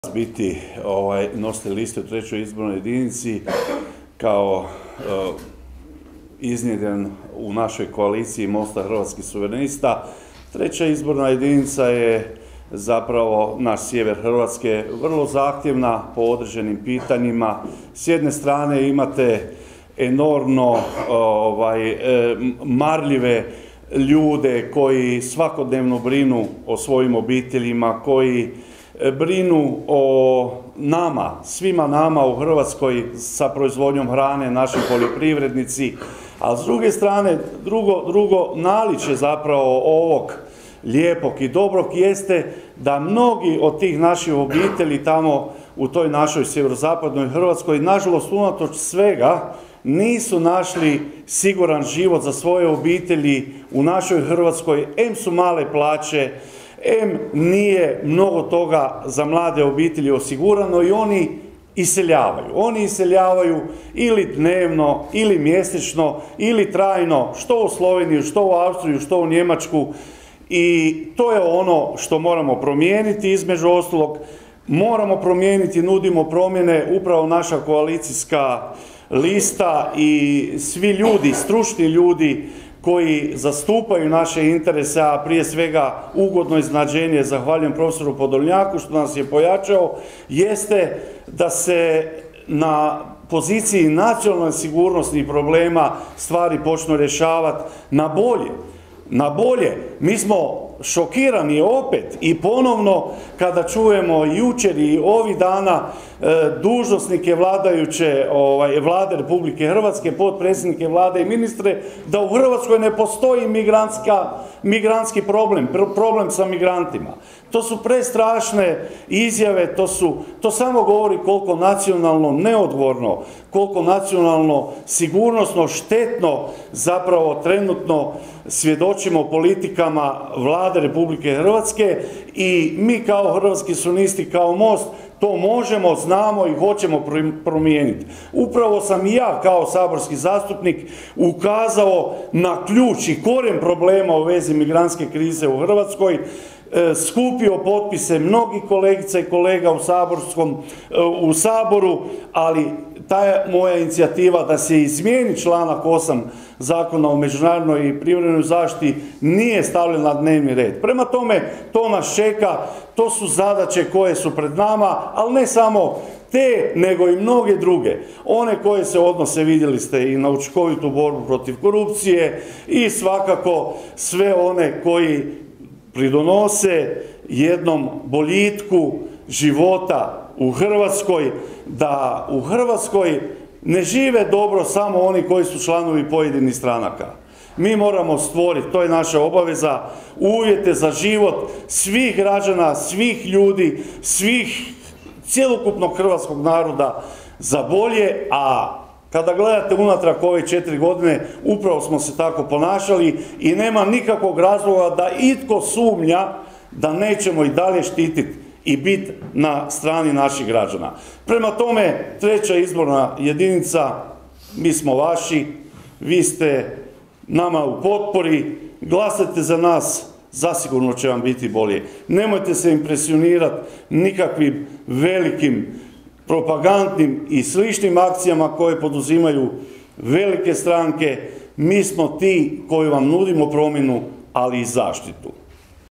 Sačić: "Mi vam nudimo promjenu ali i zaštitu"
Na konferenciji za medije održanoj u sjedištu stranke Most je predstavio nositelje izbornih lista za parlamentarne izbore.